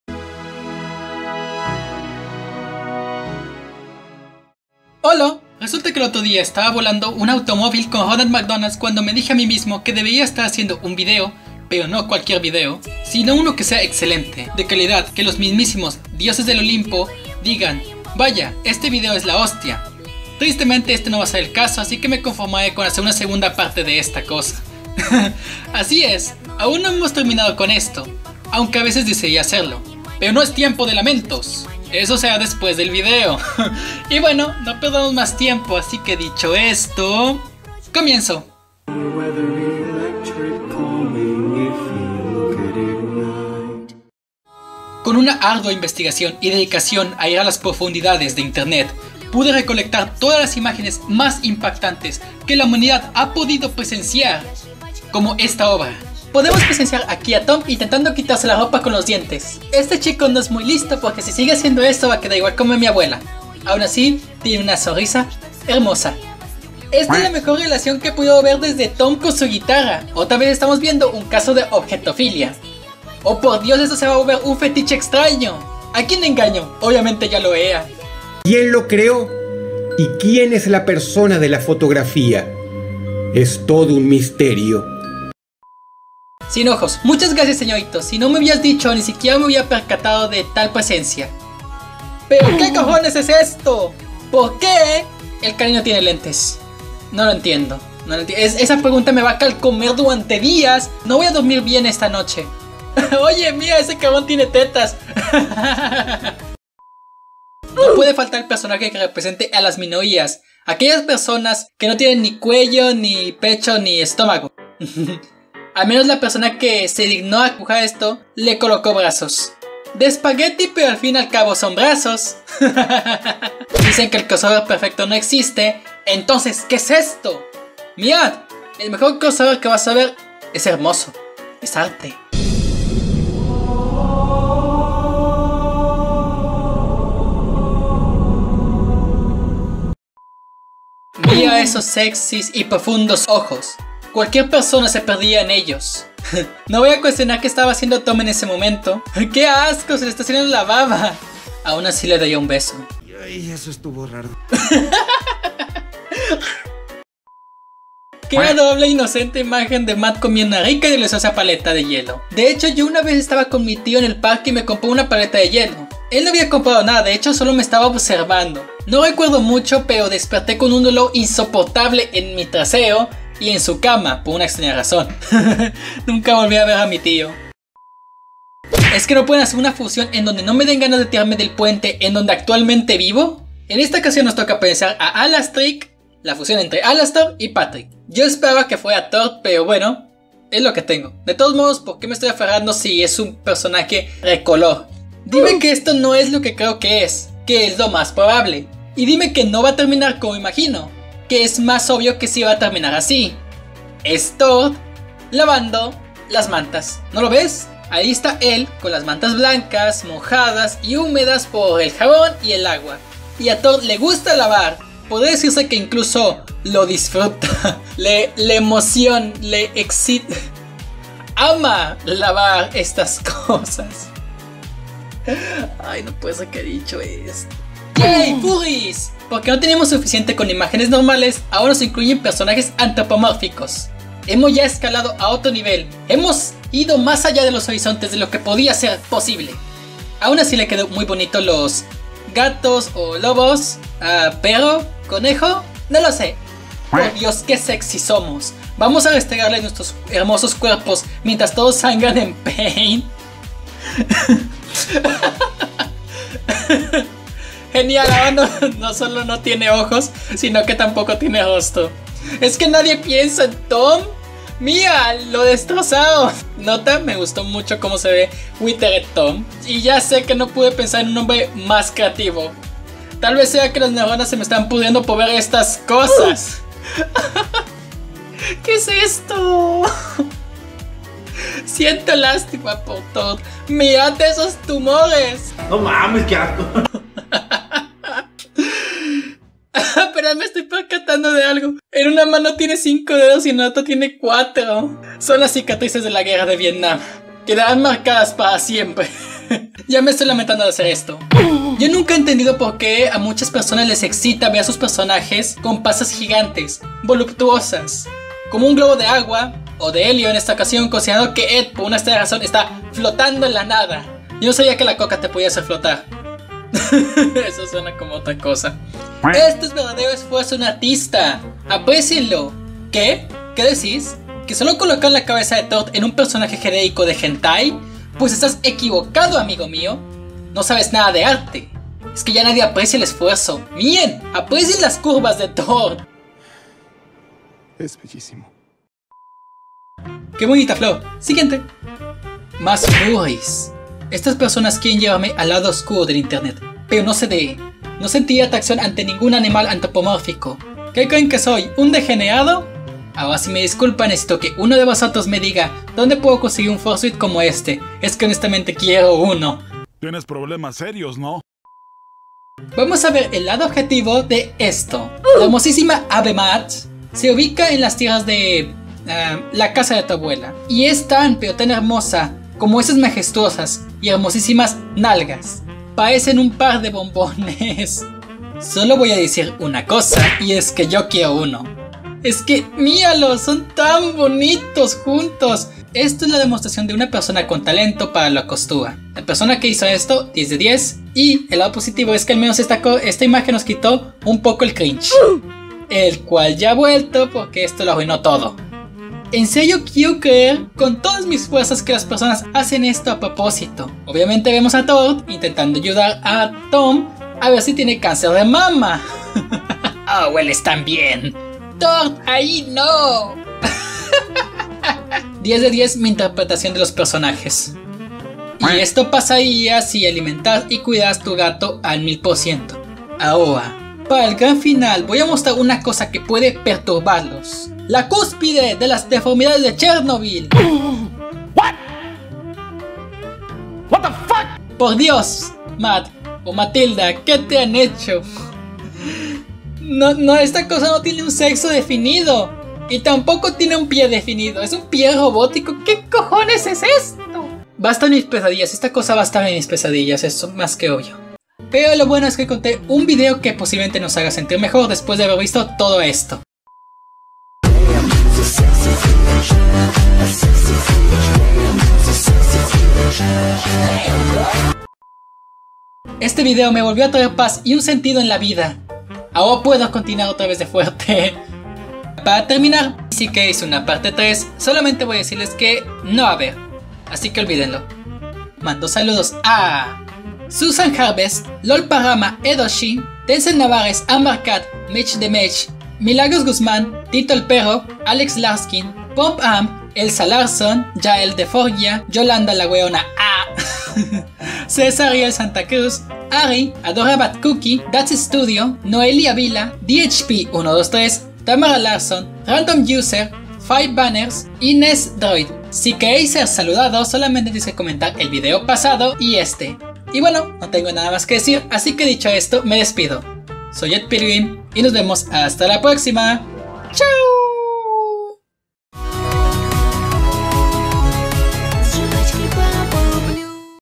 Analizando por segunda vez imágenes random de Eddsworld
ya no haré una parte tres ya que no quiero explotar esta saga, pero el lado "positivo" es que al estar en cuarentena seguiré subiendo videos seguidos, o al menos lo intentare. bueno espero que todo este bien😃
Amino (fnaf español y eddsworld español): siempre seré Edd Pilgrim